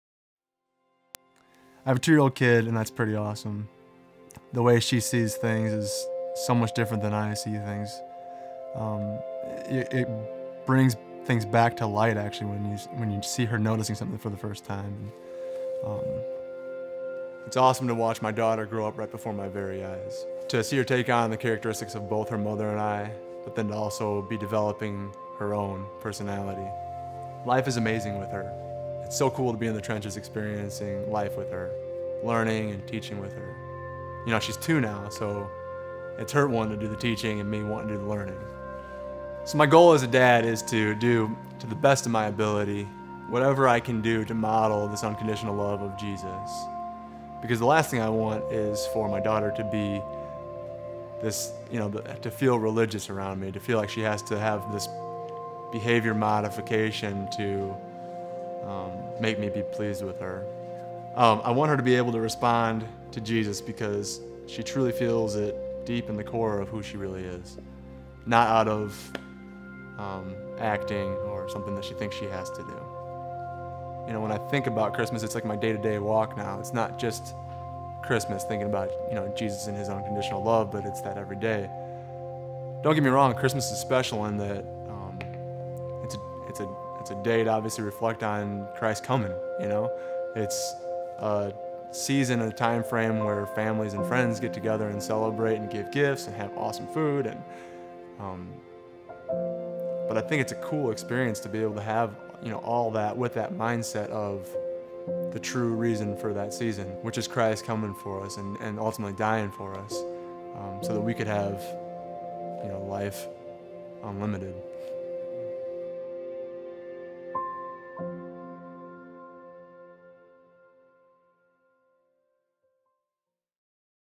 The true meaning of Christmas.
Matt is a new father and sees Gods love working through his daughter and wife.